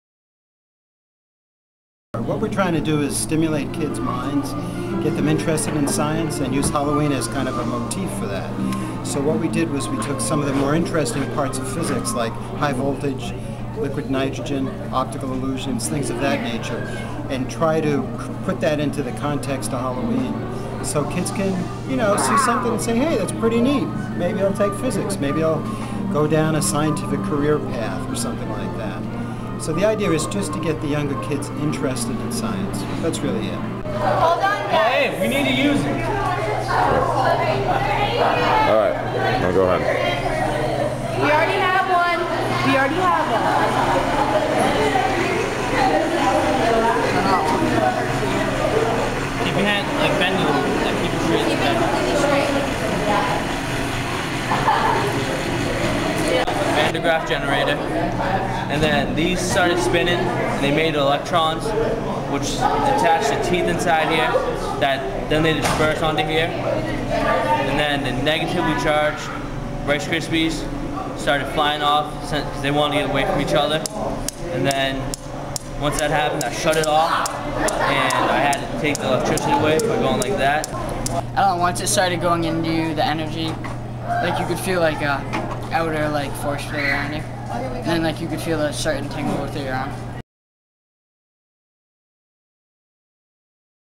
Haunted Physics Lab at Hudson High
Students participated in a Halloween-inspired science fair at Hudson High School on Oct. 25, 2007.